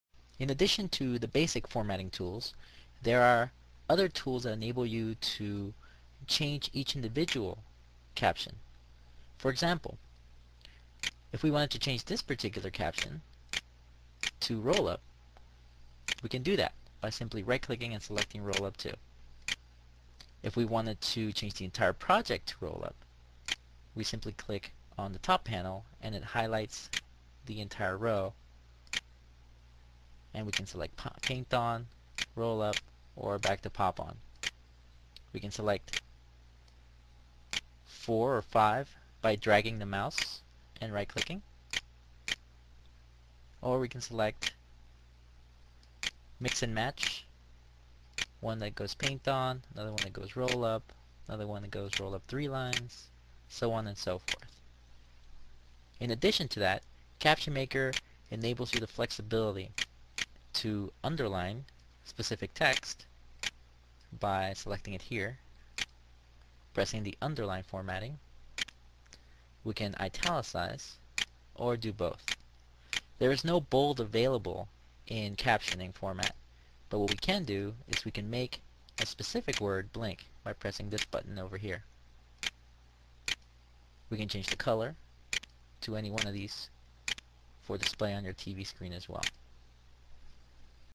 CaptionMaker - Formatting Text Advanced
When you import a raw text file into the CPC software, the software breaks the text into captions according to your specifications, e.g.,

    Number of characters per line
    Number of lines per caption
    Number of words per minute (WPM)
    Start a new caption for a new sentence or phrase (using grammar and/or punctuation)